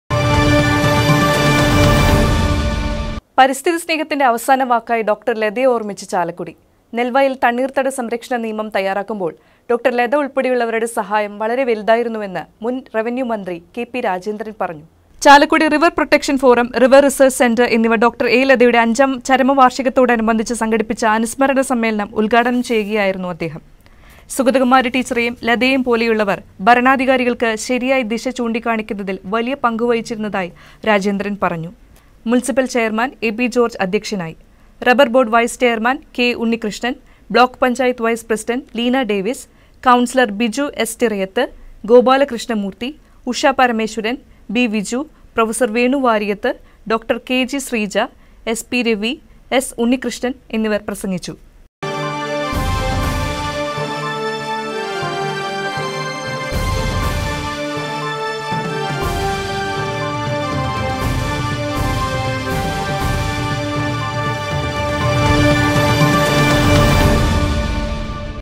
പരിസ്ഥിതിപ്രവര്‍ത്തക ഡോ. ലതയെ ഓര്‍മ്മിച്ച് ചാലക്കുടി Newsleader
ചാലക്കുടി റിവര്‍ പ്രൊട്ടക്ഷന്‍ ഫോറം, റിവര്‍ റിസര്‍ച്ച് സെന്റര്‍ എന്നിവ ഡോ. എ. ലതയുടെ അഞ്ചാം ചരമവാര്‍ഷികത്തോടനുബന്ധിച്ച് സംഘടിപ്പിച്ച അനുസ്മരണ സമ്മേളനം ഉദ്ഘാടനം ചെയ്യുകയായിരുന്നു അദ്ദേഹം. സുഗതകുമാരി ടീച്ചറെയും ലതയെയുംപോലുള്ളവര്‍ ഭരണാധികാരികള്‍ക്ക് ശരിയായ ദിശ ചൂണ്ടിക്കാണിക്കുന്നതില്‍ വലിയ പങ്കുവഹിച്ചിരുന്നതായി രാജേന്ദ്രന്‍ പറഞ്ഞു.മുനിസിപ്പല്‍ ചെയര്‍മാന്‍ എബി ജോര്‍ജ് അധ്യക്ഷനായി